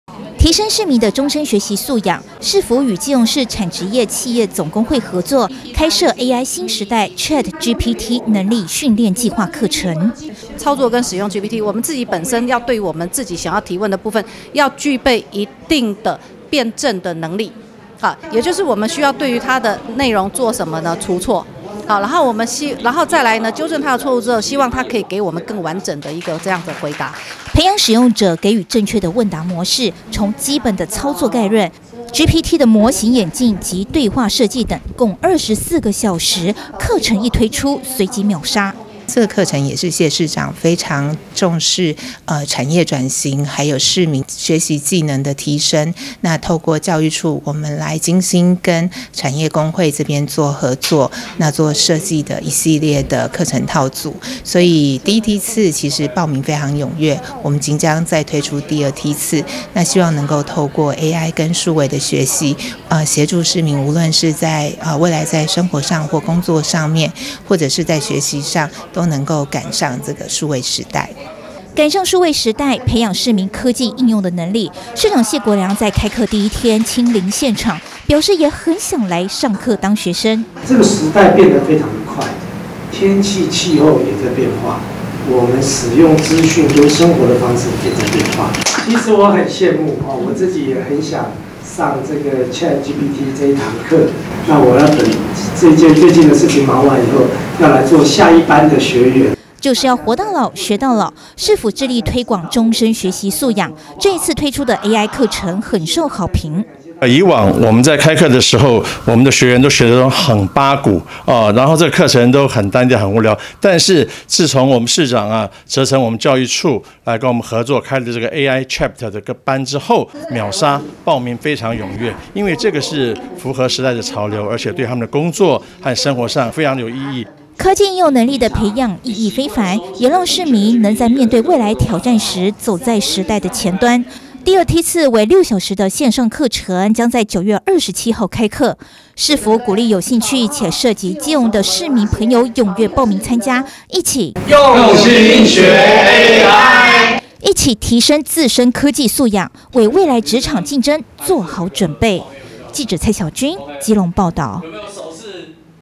數位時代|媒體素養|迎接AI新時代 基隆推ChatGPT能力培訓
（中嘉新聞記者 蔡曉君/基隆採訪報導）基隆市政府為提升市民媒體素養並推動產業轉型，特別與基隆市產職業企業總工會合作，共同舉辦「AI新時代，ChatGPT能力訓練計畫」課程。基隆市長謝國樑親自到場給參與學員們鼓勵與肯定。

因應教育部於112年發佈的「數位時代媒體素養教育白皮書」，基隆市政府除了在學校教育方面積極投入資源外，更開放本課程給各界及各年齡層市民參加，以推廣終身學習。課程內容豐富紮實，包括基本操作概論、Open AI介紹、GPT模型演進及有效對話設計等，共計24小時。

謝國樑市長在始業典禮上強調，基隆市政府一直致力於提升市民的終身學習素養，尤其重視科技應用能力的培養，以確保市民能在面對未來挑戰時走在時代前端。

市府已規劃於9月底加開第二期課程，分為兩梯次：第一梯次為實體課程，將於9月19日至26日舉行；第二梯次為6小時的線上課程，將於9月27日開課。市府鼓勵有興趣且設籍基隆的市民踴躍報名參加，提升自身科技素養，為未來職場競爭做好準備。報名日期為即日起至9月16日，詳情可洽詢基隆市產職業企業總工會（電話：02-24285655）。